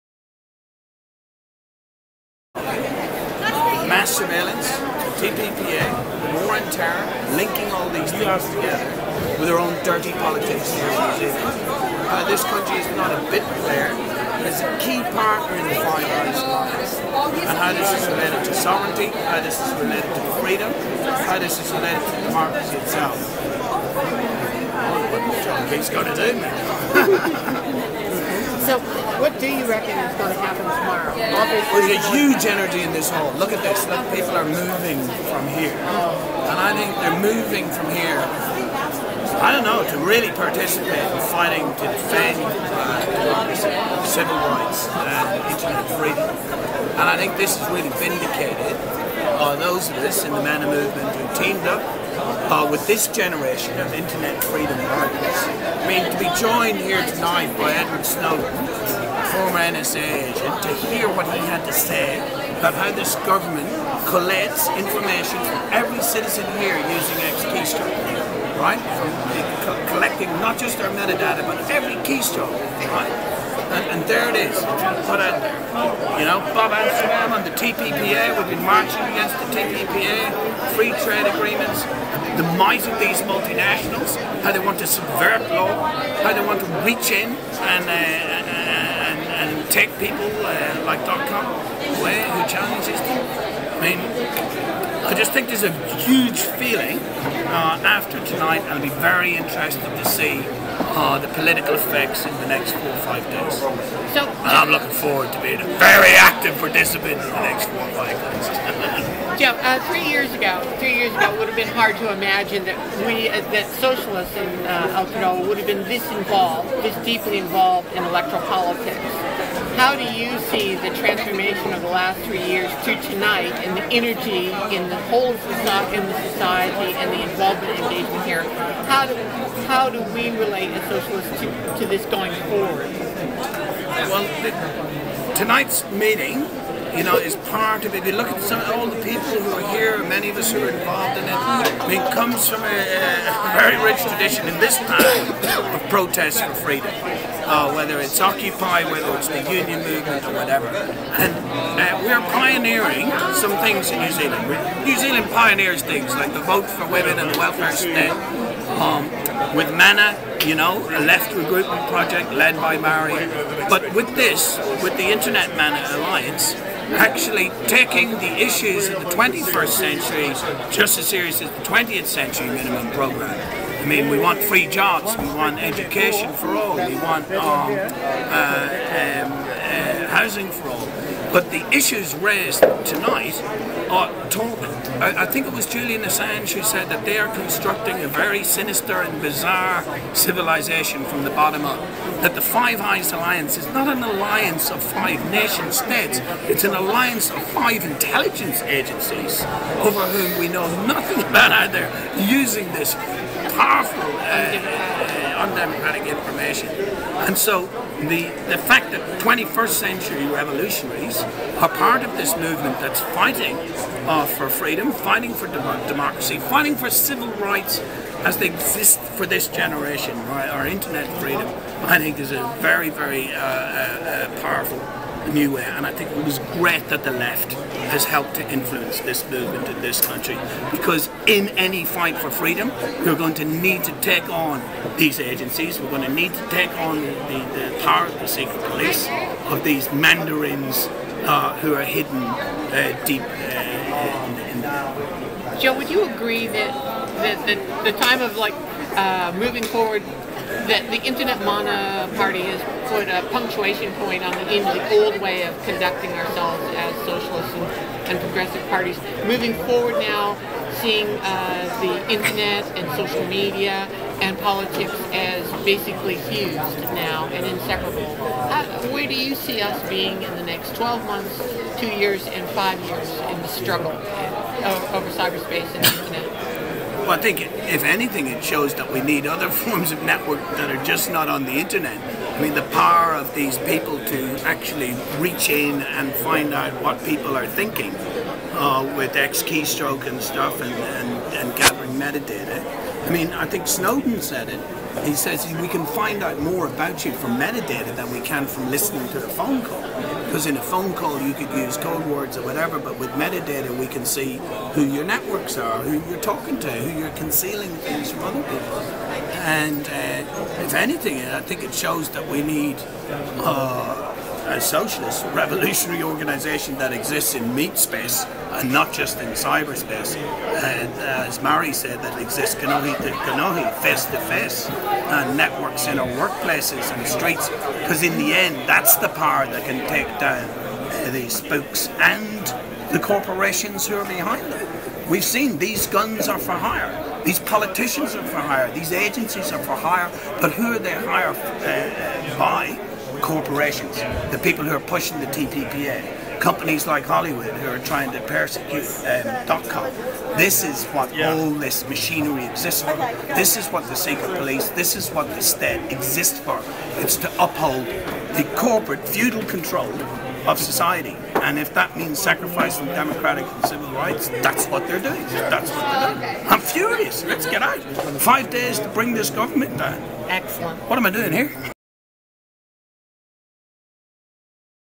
New Zealand spy scandal: 'Five days to bring this government down'
Auckland Town Hall was packed to overflowing on September 15, with almost 2000 people gathered to hear surprise guest, US National Security Agency (NSA) whistleblower Edward Snowden, speak over the Internet.

Snowden address the crowd about evidence the New Zealand government has - despite its repeated denials - been collaborating with US authorities to carry out wholesale surveillance and data collection on NZ citizens.

This interview is with Joe Carolan - an InternetMANA candidate - conducted straight after the meeting finished. Carolan is also a member of Socialist Aotearoa.